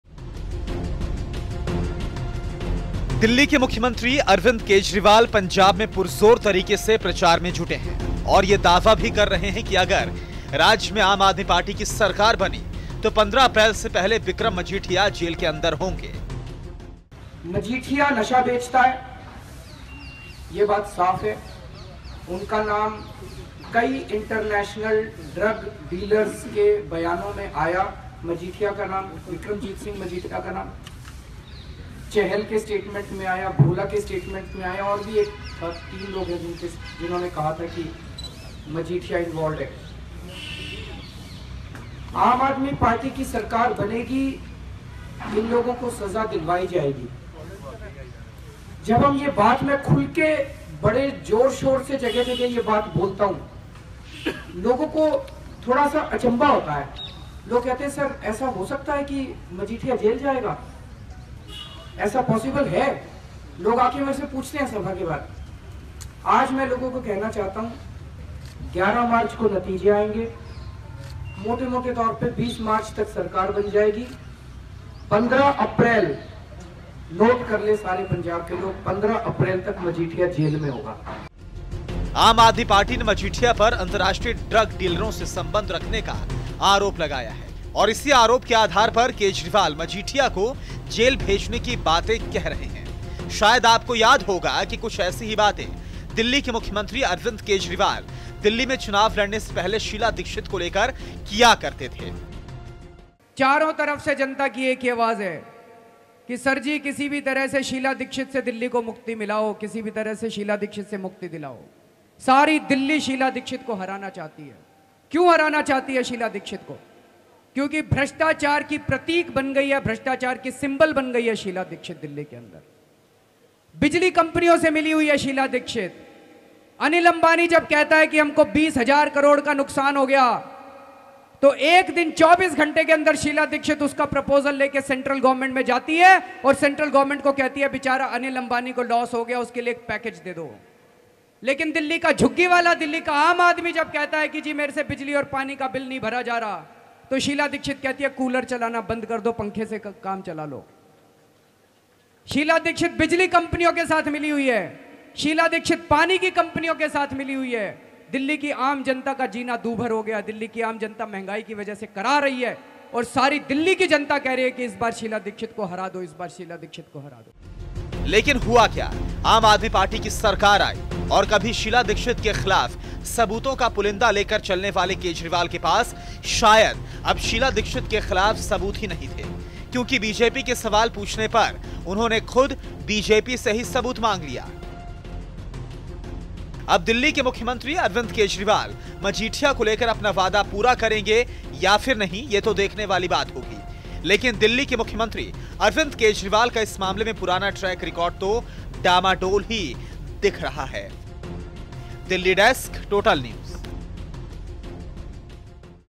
Arvind kejriwal says, ’Vikramjeet Majithia’ will be in jail till 15th April 2017
January 21: During a press briefing Delhi CM Arvind Kejriwal claimed Vikramjeet Majithia ( leader of Akali Dal ) will be in jail till 15th January 2017 as the "Drug Mafia" of Punjab if he forms government in Punjab.
Earlier kejriwal claimed similar things for Sheela Dixit during the time of Delhi elections.But CM took a clear u-turn, will statements be accessible this time.